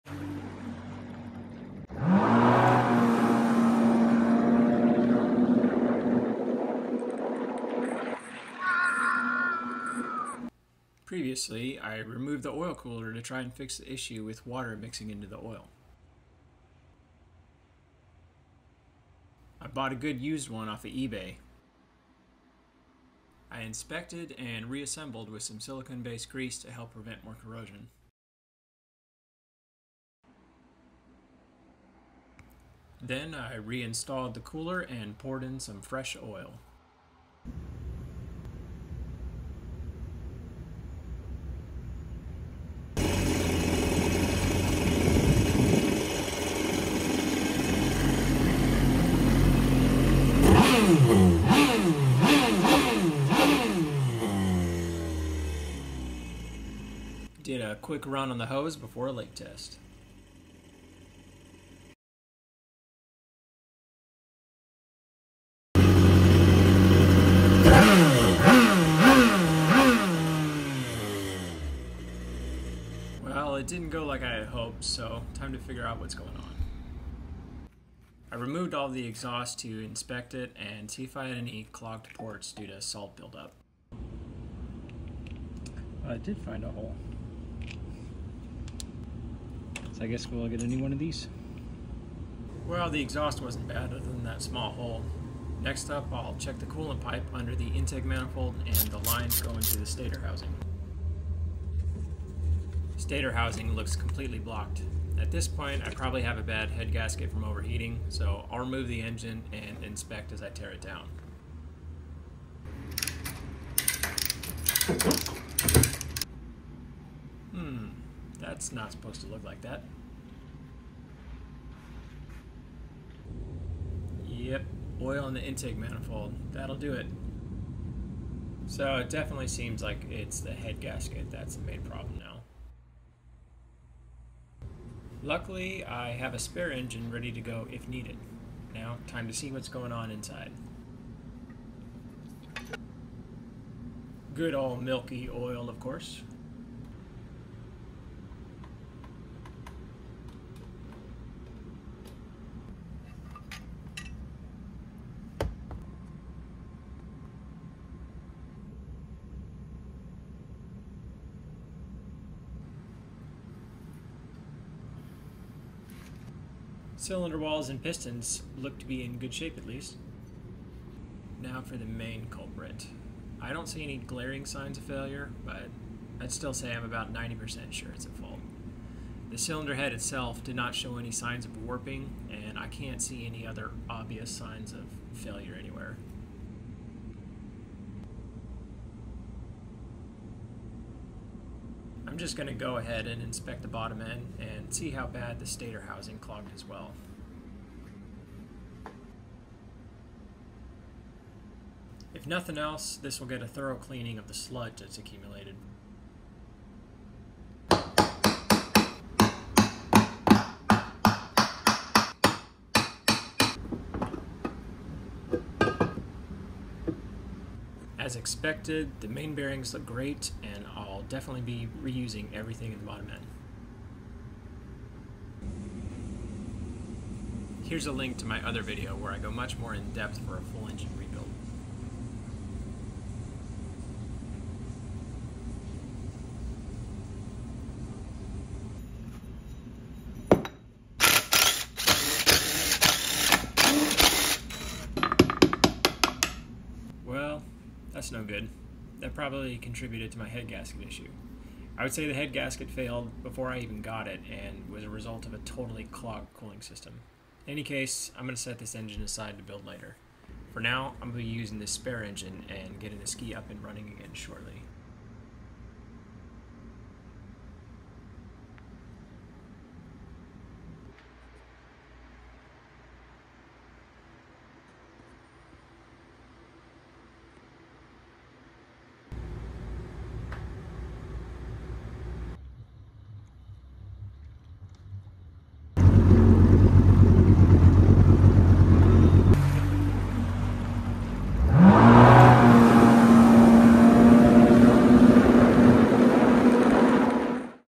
Part 2 Repair Jet Ski Oil Contamination
I go through a 2019 Kawasaki STX-15F and find out why water is getting into the oil.